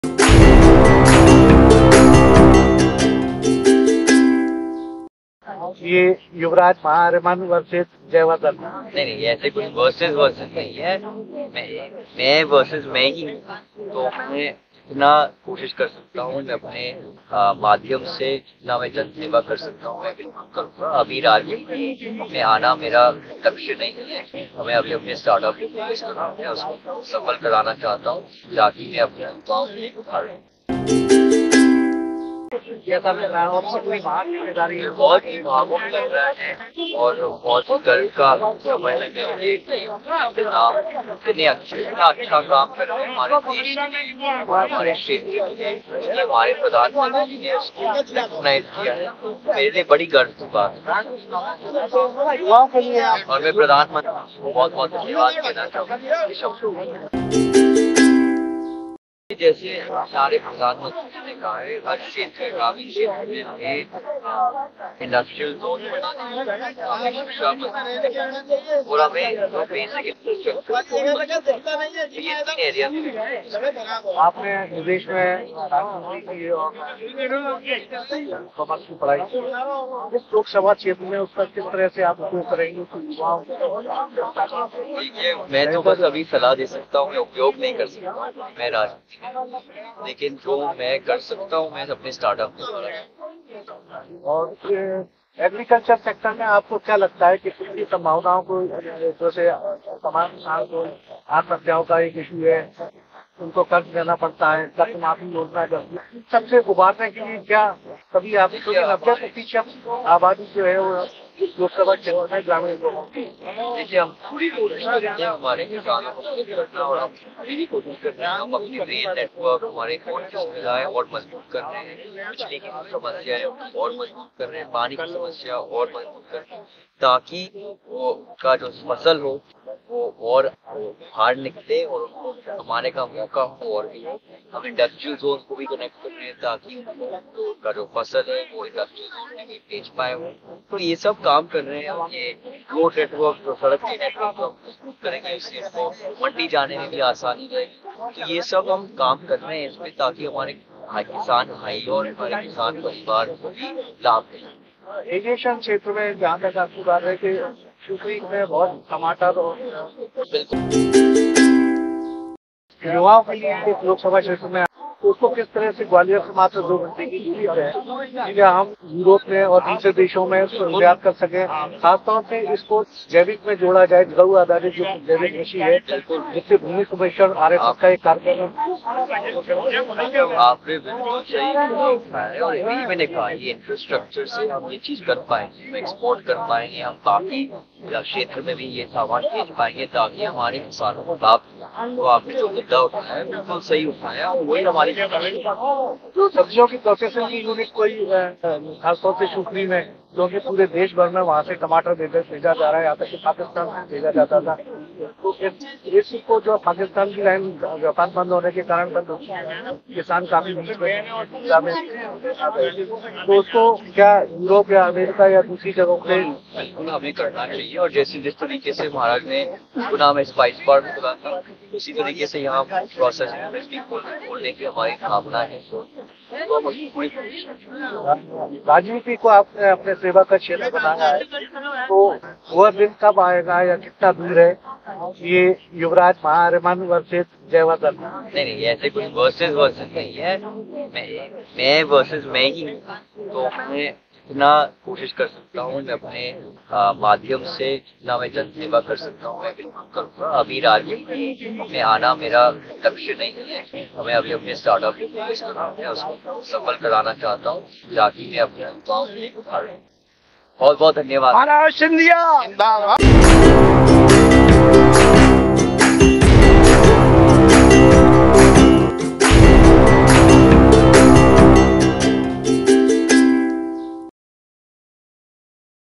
शिवपुरी में फूड प्रोसेसिंग यूनिट का करेंगे प्रयास युवराज
अभी राजनीति में आना लक्ष्य नहीं है
बेहतर इंफ्रास्ट्रक्चर से विदेश में टमाटर भी जा सकते हैं युवराज
गुना लोकसभा चुनाव के लिए प्रचार कर रहे युवराज  महा आर्यमन ने इंफ्रास्ट्रक्चर पर ध्यान देने का मन बना लिया है वह गुना के स्पाइस पार्क की तर्ज पर शिवपुरी में भी फ्रूट प्रोसेसिंग यूनिट और इंडस्ट्रियल जोन बनाने का विचार महाराज ज्योतिरादित्य सिंधिया को देंगे जिससे टमाटर  सहित  अन्य कृषि जिंसों कीपहुंच न केवल देश दूर दराज के एरिया में बल्कि विदेश तक पहुंचा जा सकता है इंफ्रास्ट्रक्चर में अधिक से अधिक ध्यान देकर शिवपुरी से महज 110 किलोमीटर दूर ग्वालियर एयरपोर्ट से  टमाटर के विदेश निर्यात की संभावनाओं को भी तलाश किया जा जाएगा
जिससे किसानों की आमदनी मे  बढ़ोतरी हो जाएगी

   महा आर्यमन वर्सेस जयवर्धन जैसी कोई चीज नहीं है में वर्सिज में ही हूं मेरा किसी से भी मुकाबला नहीं है मेरा मुकाबला मुझ से ही है